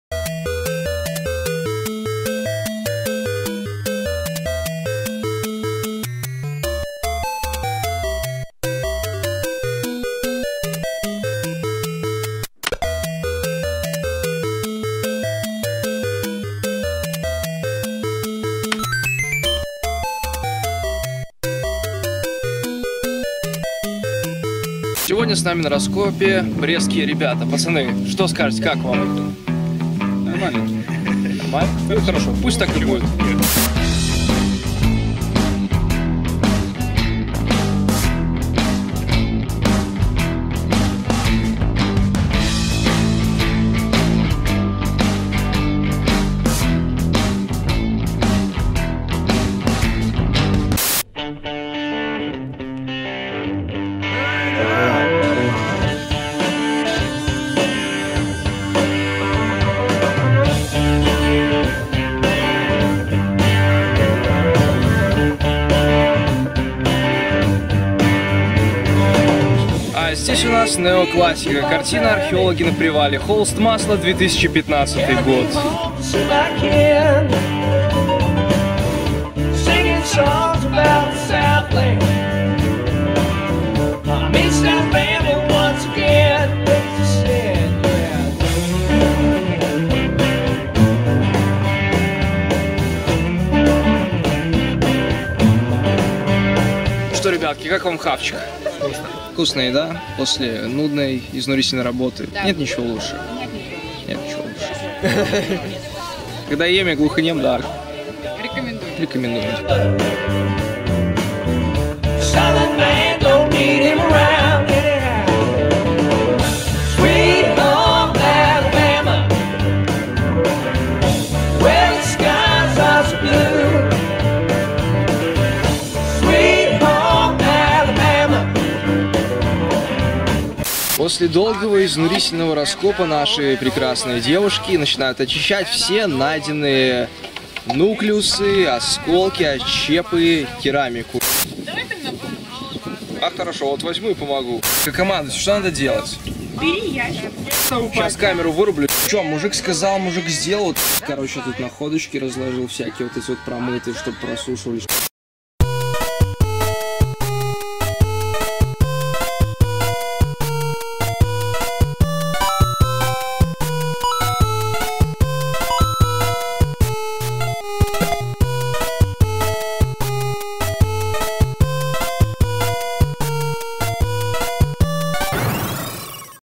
Archaeology: Kamenuki. Часть 3
Часть 3. Рабочий день студента-практиканта. Совки и лопаты. Нивелир и находки. Бутерброды с салом.
Фильм рассказывает о буднях и праздниках Археологической экспедиции БГУ, в 2015 г. проводившей раскопки в окрестностях деревни Каменюки (Каменецкий район Брестской области).
Сюжет подготовлен студентом исторического факультета Денисом Силенковым.